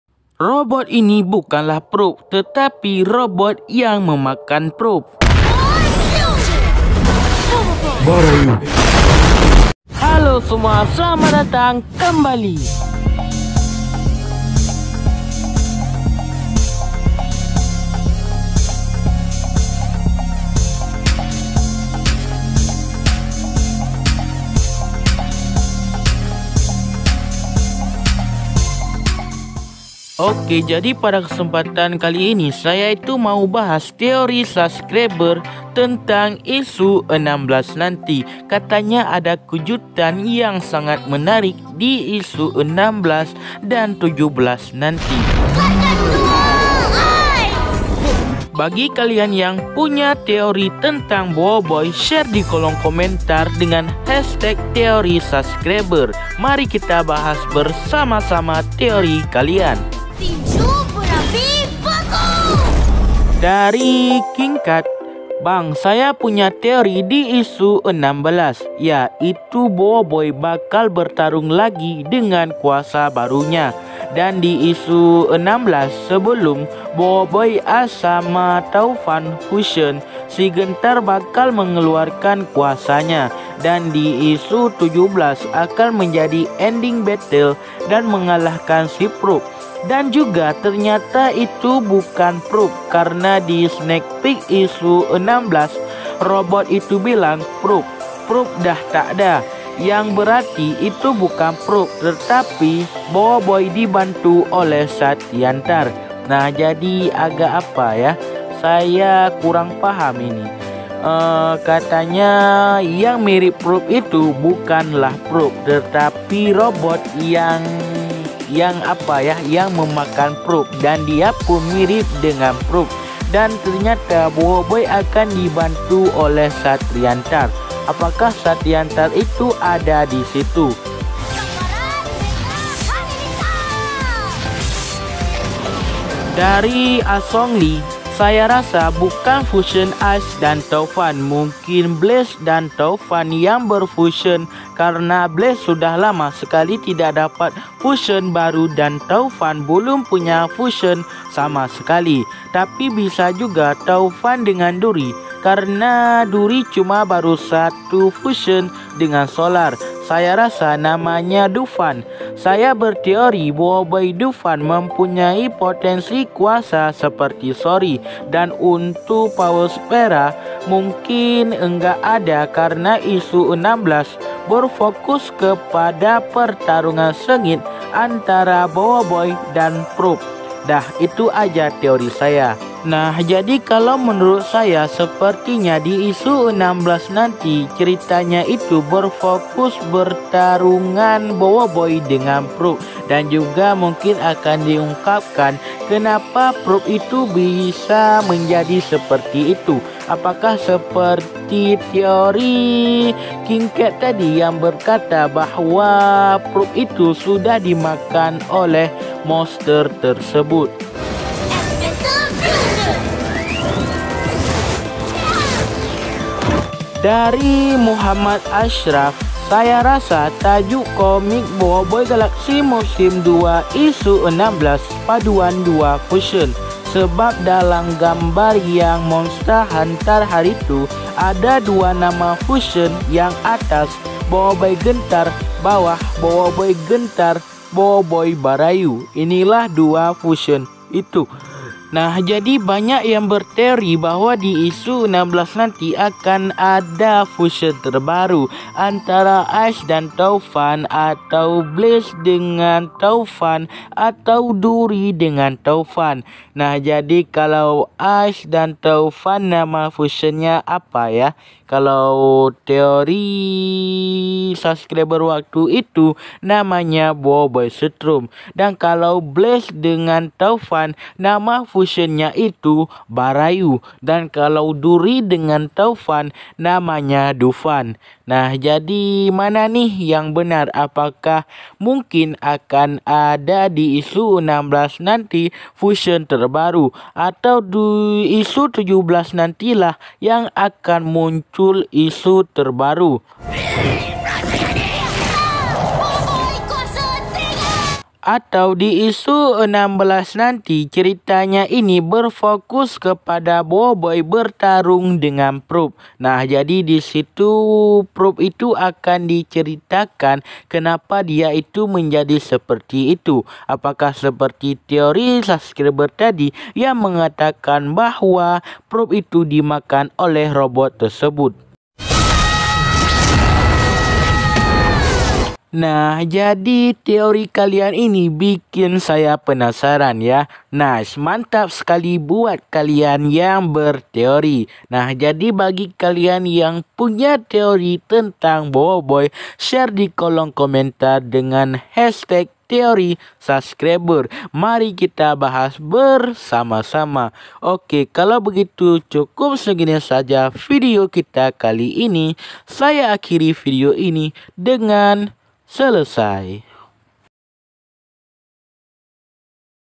bahas komik boboiboy galaxy musim2 isu 16 fusion baru
bahas komik boboiboy galaxy musim 2 isu 16 dan 17 katanya ada fusion.

dan ternyata robot jahat di isu 15 bukan probe tapi robot yang makan probe dan mirip probe hmmmmm.

ice + topan = boboiboy setrom
blaze + topan = boboiboy barayu
duri + topan = boboiboy Dufan

0:00 cuplikan dan intro
0:29 bahas teori subscribe isu 16
1:02 teori subscriber pertama
1:28 Probe dah tak de
2:14 teori subscriber ke2 fusion baru
3:35 teori subscriber ke 3
4:09 prediksi 3 fusion baru topan
4:57 fokus cerita isu 16
5:24 nice mantap teori kalian semua

_______ ayo beli guys _______